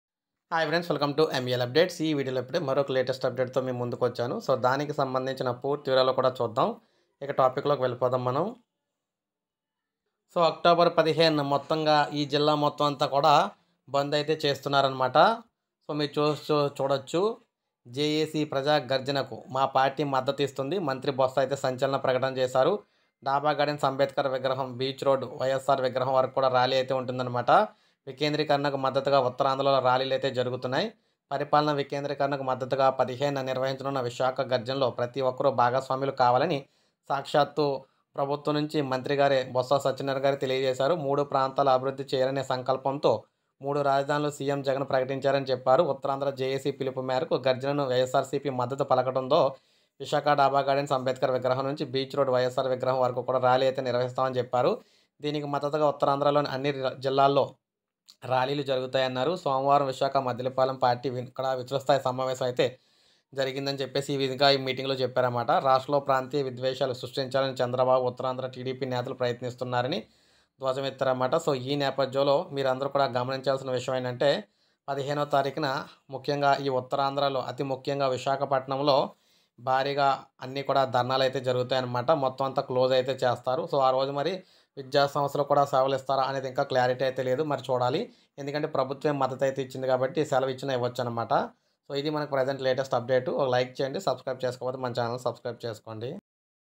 అక్టోబర్ 15 ఇక్కడ మొత్తం బంద్ చేస్తున్న ప్రభుత్వం latest update on bhandh on 15th October.
hello everyone today update is AP government announced that bhandh on 15th October In AP .@malupdates